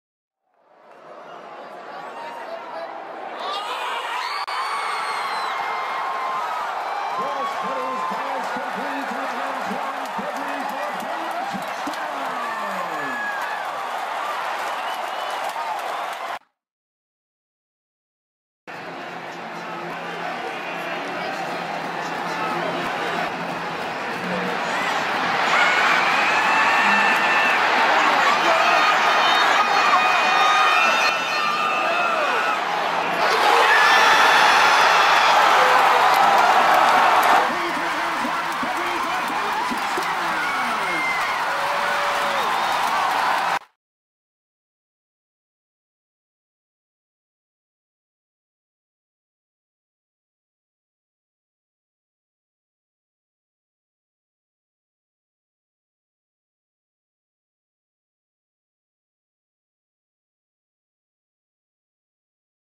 Fantastic Pass and Catch by Baylor Quarterback Bryce Petty and Reciever Antwan Goodley
Bryce Petty makes a great throw to Antwan Goodley who catches it.